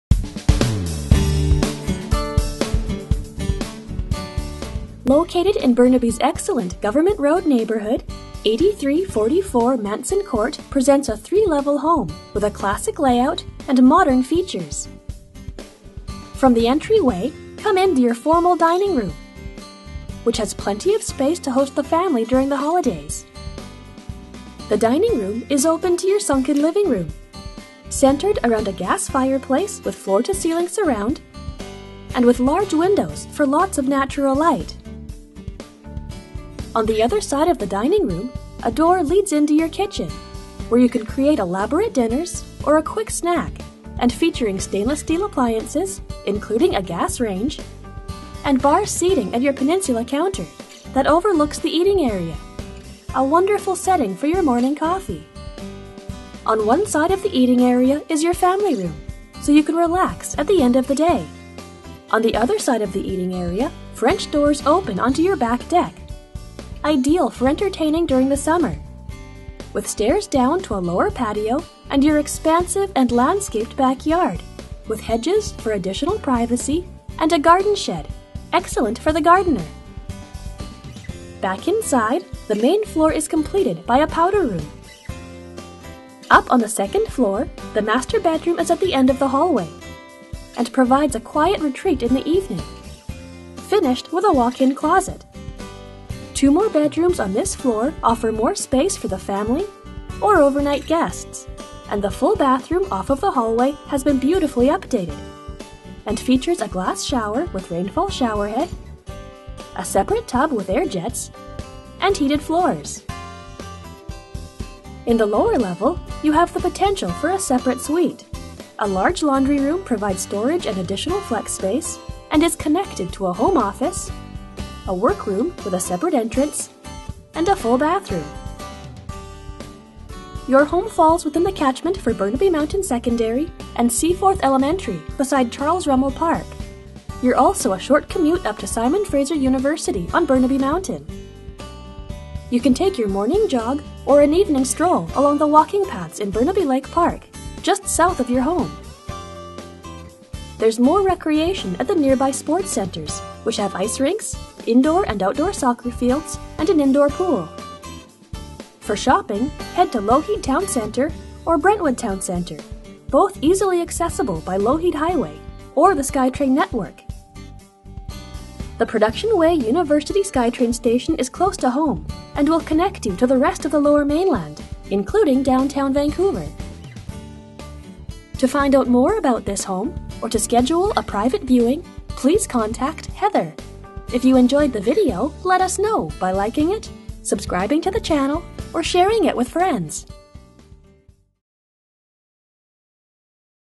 8344 Manson Court, Burnaby
Pride of ownership shows throughout this beautiful one owner custom built home. Situated on a 12,232 sq.ft. level lot on a quiet cul-de-sac in prestigious Gov't Road area. An area where celebrities are proud to call home. Great layout on 3 floors featuring 3 bdrms, 3 baths, family room off kitchen, sunken living room, dining room, laundry room & summer kitchen. Plenty of storage throughout including a charming 203 sq.ft. shed located in the beautiful landscaped garden. Many updates include roof (2013), windows (2012), hot water tank (2015), etc. Walking distance to Skytrain Station, Costco & Lougheed Mall. Close to all levels of schools - Seaforth Elementary, BBY Mountain Secondary & SFU.